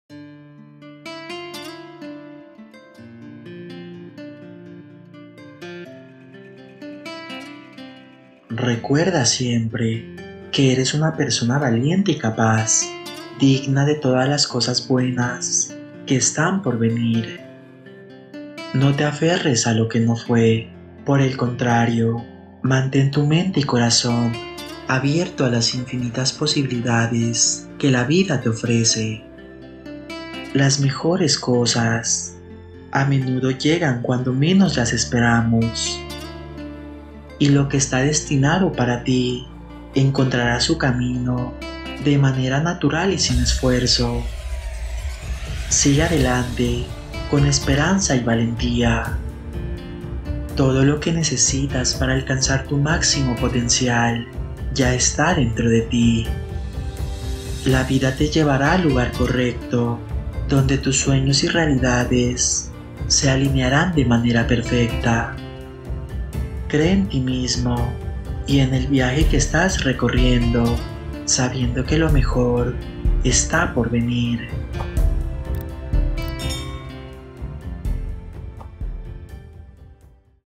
PARA TI UN HERMOSO MENSAJE Ábrelo Escúchalo y Comparte Estas BELLAS FRASES MOTIVADORAS y POSITIVAS
Para ti bonitos mensajes de la vida con hermosas palabras motivadoras. Lindo video motivacional, Reflexion, Reflexiones de vida, Frases, Amor propio y Mensajes de Dios que te ayudaran a superar cualquier desafío que le vida te ponga por delante. DIOS TE BENDIGA SIEMPRE.

GRACIAS por tu valioso like, por tus comentarios y por compartir el video.